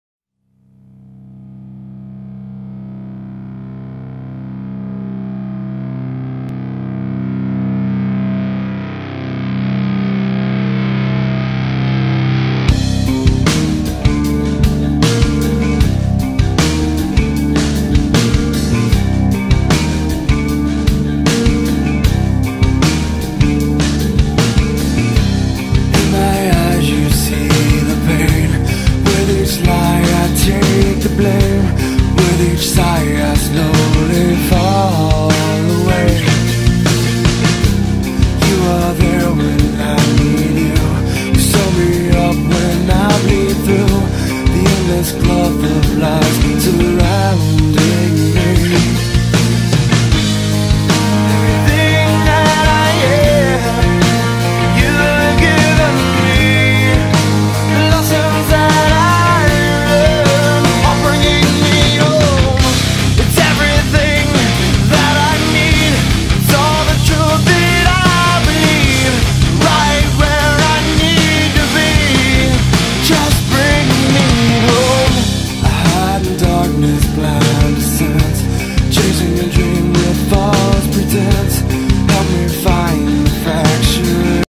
Jonker Light at Port of Brisbane (with music)
Track-based Jonker Lighting Tower working night shift at the Port of Brisbane.